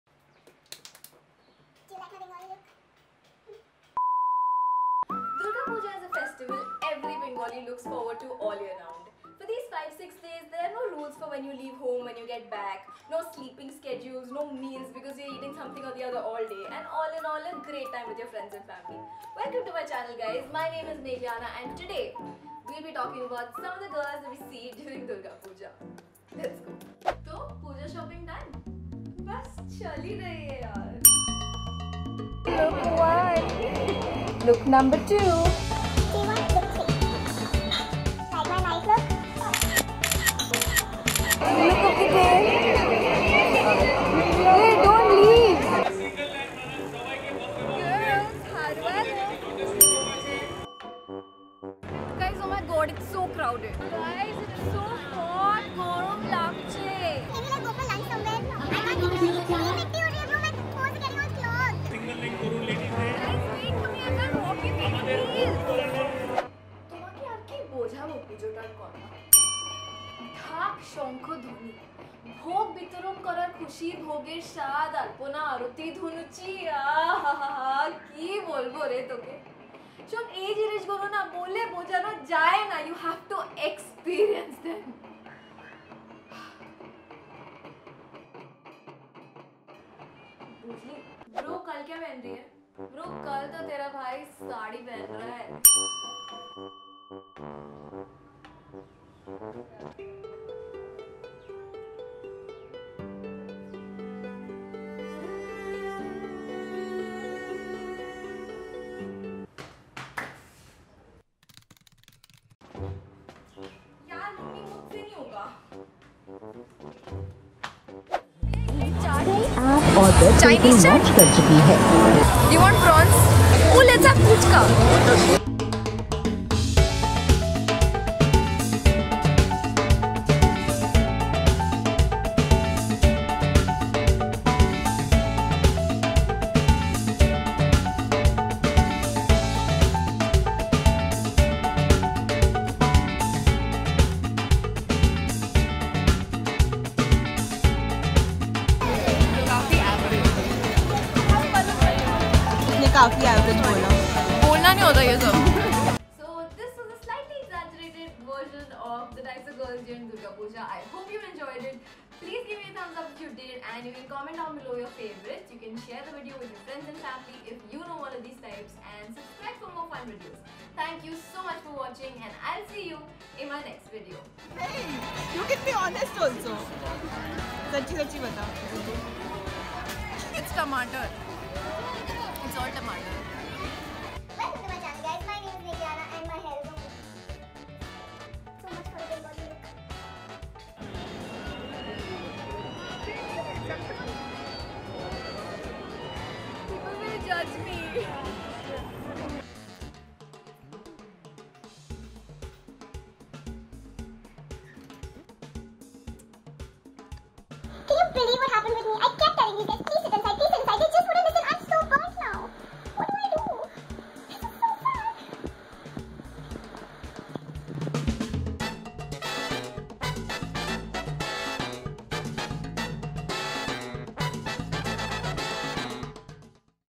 TYPES OF GIRLS DURING DURGA PUJA | Meghlayana Banerjee
Happy festive season everybody!
This year I thought it would be fun to film a Durga puja themed video. All jokes, nothing serious, I hope you enjoy watching as much as I enjoyed making it.

xoxo